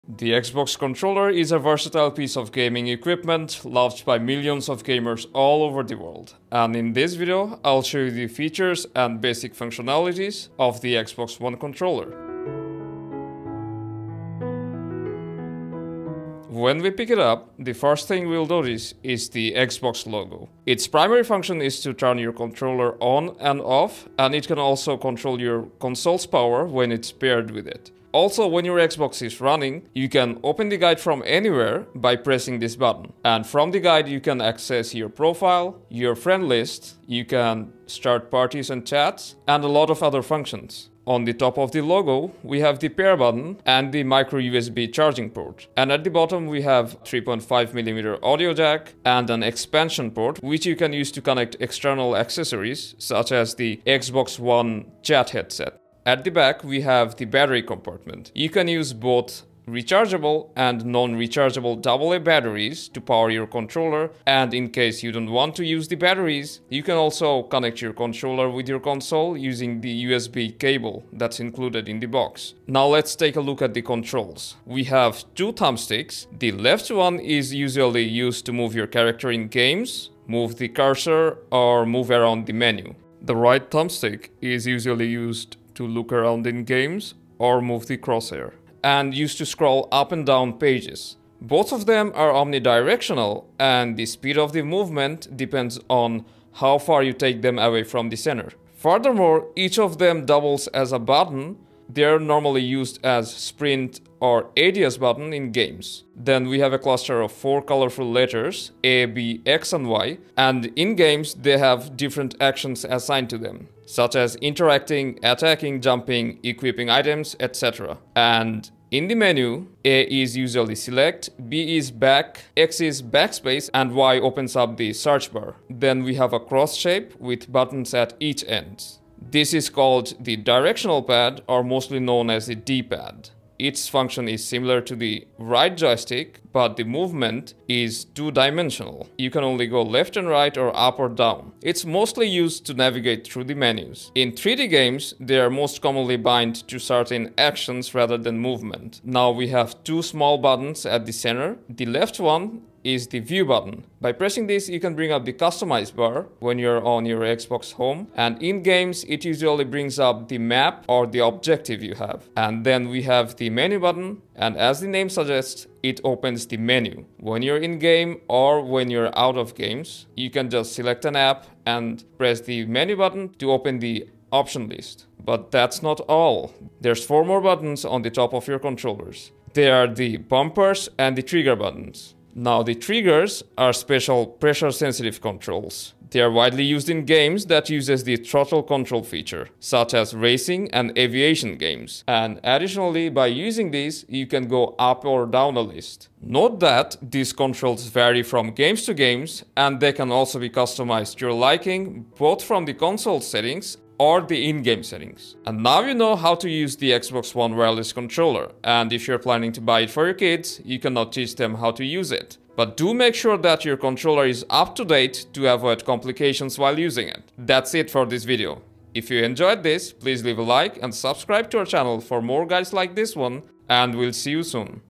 Xbox One S Wireless Controller : How To Use Beginner’s Guide!
Just got your Xbox One S and got intimated by all the buttons, thumb-sticks, triggers, bumpers, and ports, what they are and their functions? Here is a complete beginner's guideline about how to use the Xbox Controller and what buttons or ports function in detail.

0:00 Xbox One Wireless Controller
0:16 Buttons and Ports on the Controller
0:56 Battery and Charging of your Controller
1:12 Controls of the Controller
1:47 Colorful Letter Buttons Function
2:06 Cross Button Function
2:29 2 Small white buttons function
2:56 Bumpers and Triggers
3:16 Please Note